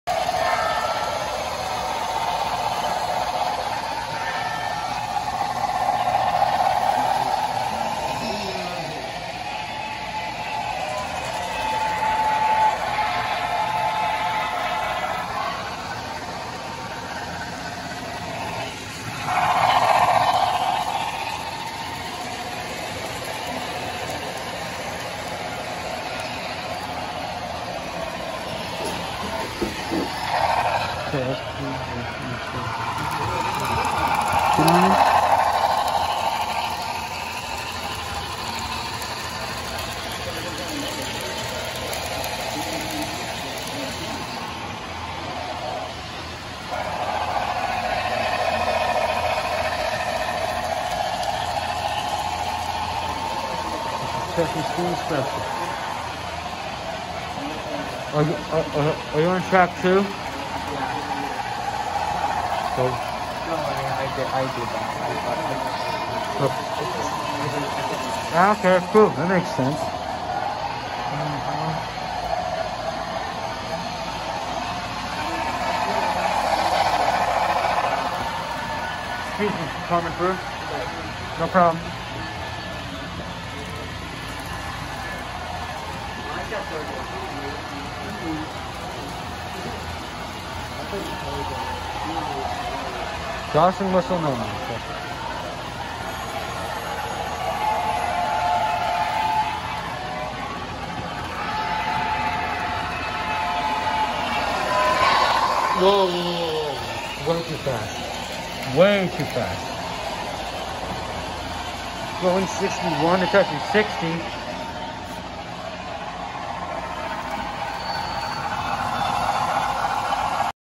Chessie steam special at the elmhurst model railroad club
Hello guys! I’m back with new train club action after a while. I apologize for the video delays. I was busy with family.

Peace ✌️ and happy new year

Rocky fanatic 68